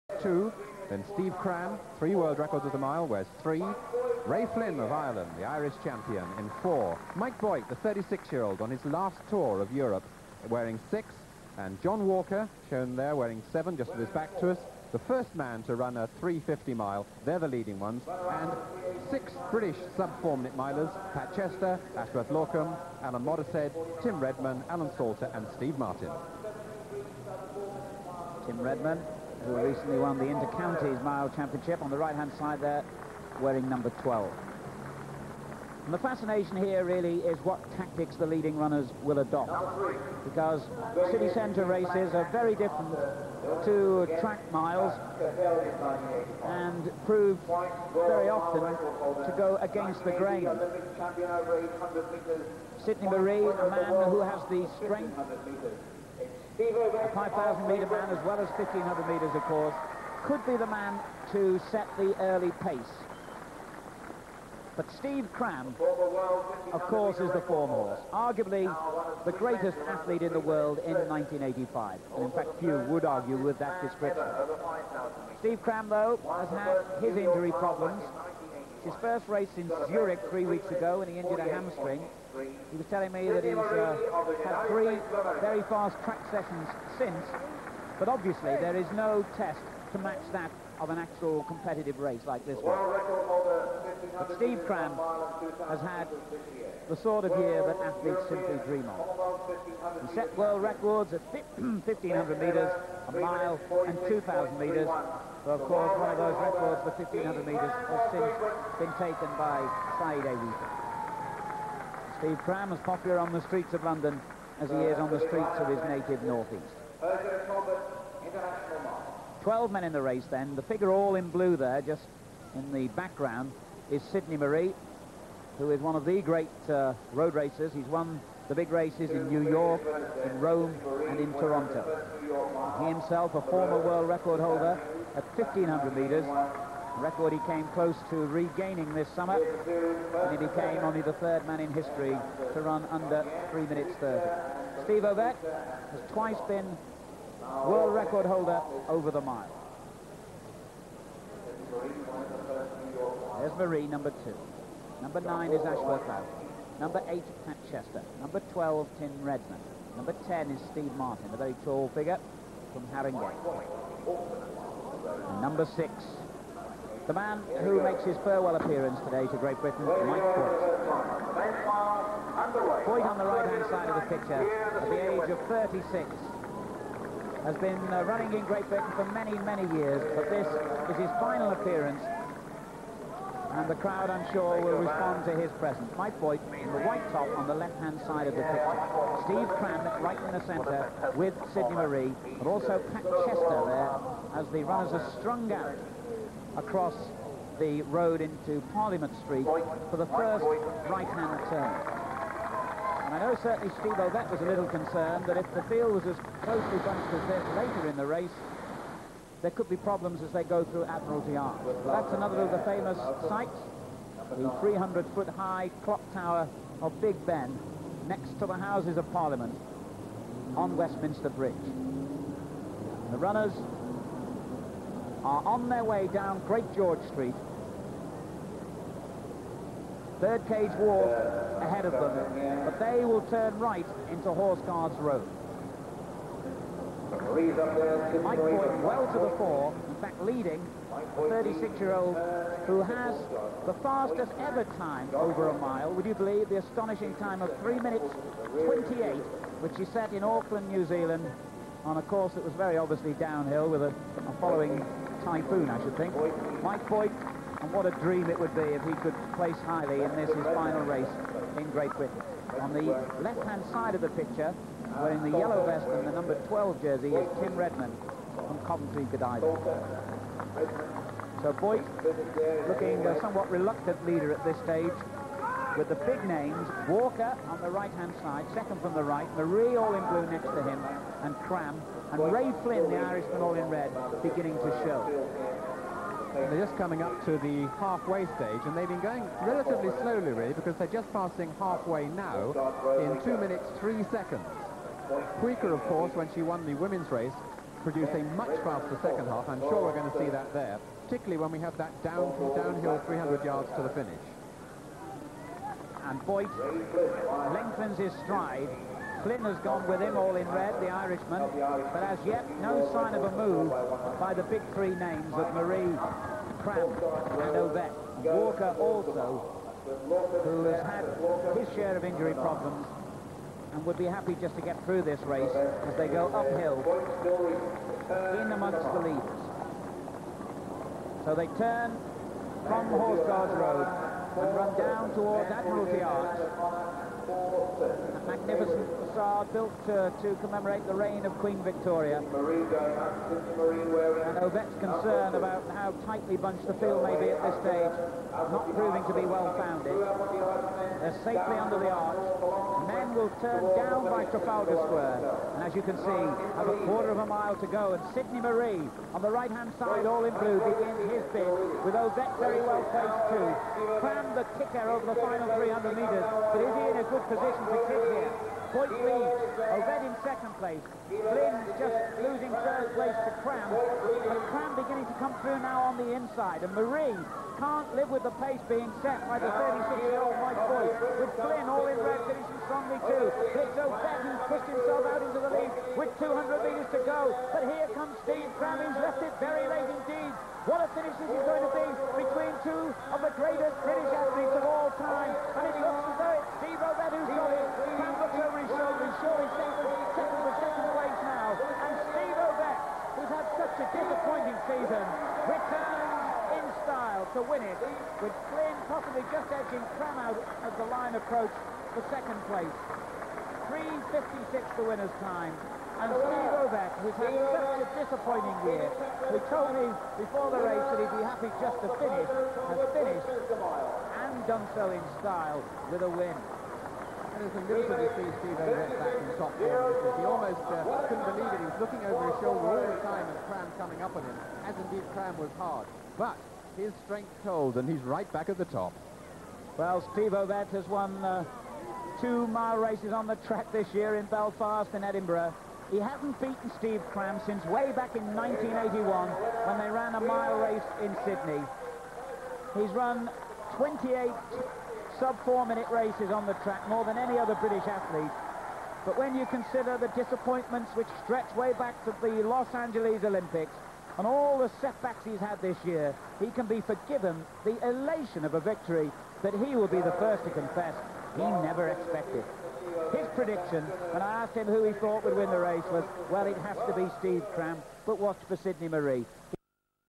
London Street Mile 1985 - Steve Ovett v Steve Cram
This race took place on the streets of London at the end of the 1985 season.
Steve Cram was at the peak of his powers, having broken 3 world records that summer, while Steve Ovett, probably past his peak over that distance, had been having a quiet year, adjusting to the problems he had faced at the LA OLympics of 1984.